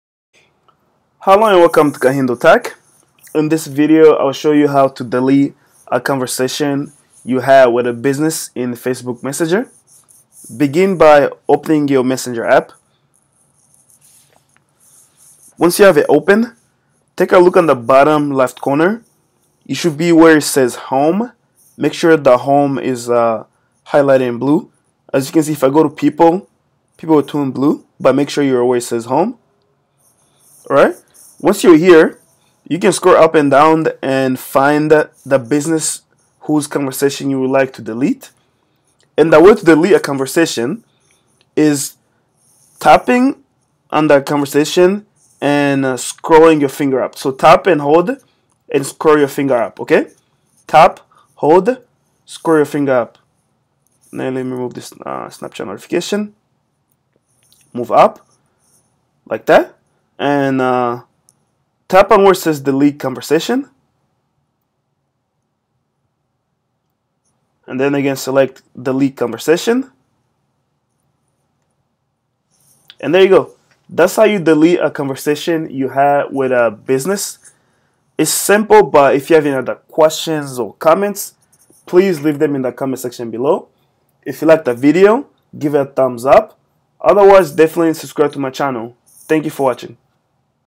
How To Delete A Conversation With A Business Facebook Messenger
This video shows How To Delete A Conversation With A Business Facebook Messenger.